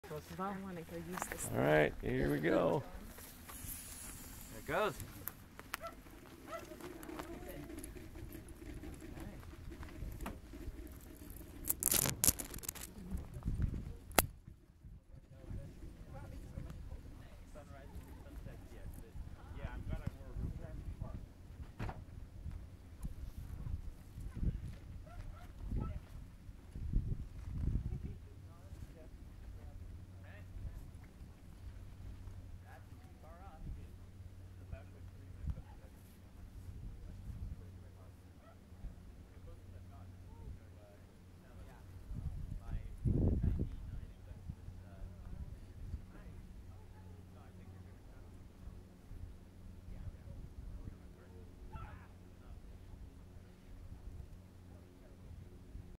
Eclipse 2017 360˚ Balloon Observation Launch
Eclipse 2017 360˚ Balloon Observation launch near Sisters Oregon